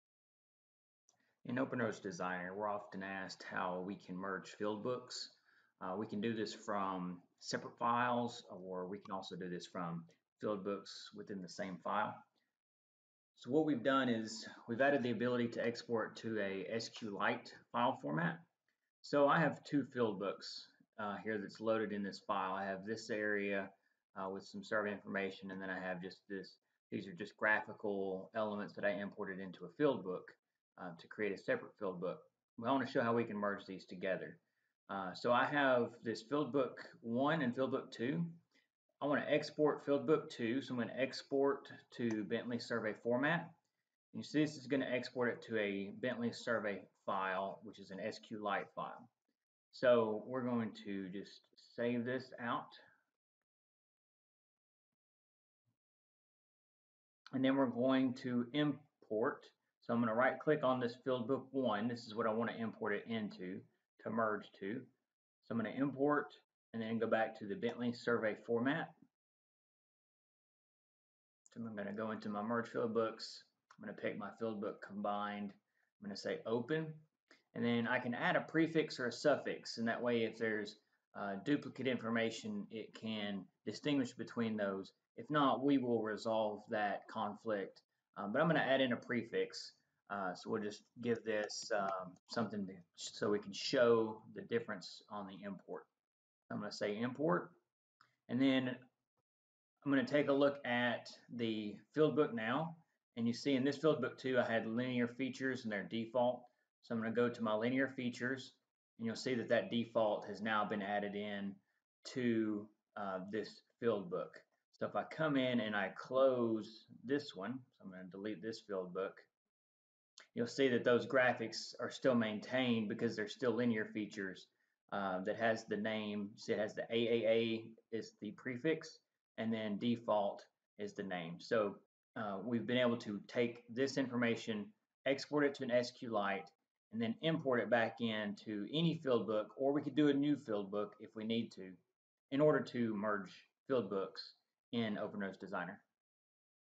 OpenRoads Designer - Merge Separate Survey Field Books
This video will demonstrate the technology available to merge and combine separate field books utilizing an OpenRoads Survey Format file (SQLite) as a file format for export and import into a field book.